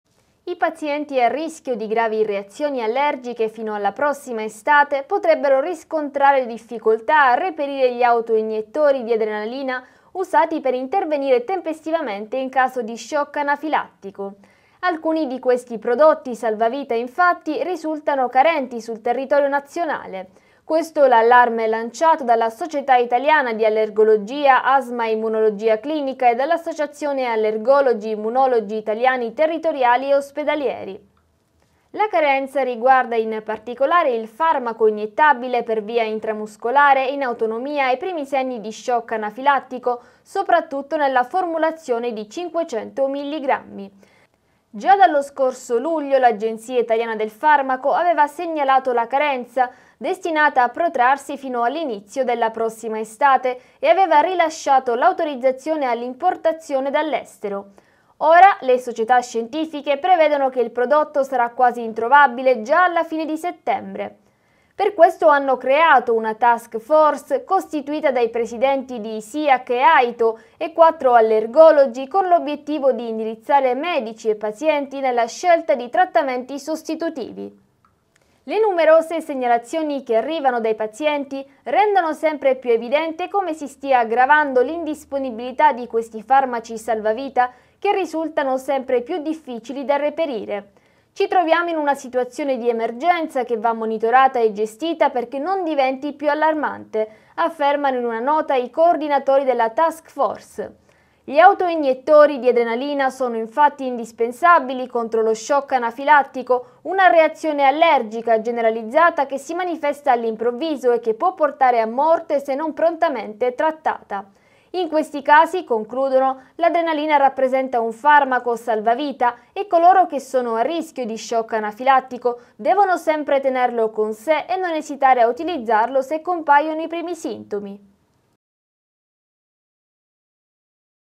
Allarme dei medici, mancano i farmaci anti-shock anafilattico
Allarme dei medici, mancano i farmaci anti-shock anafilattico: "Auto-iniettori di adrenalina carenti fino alla prossima estate"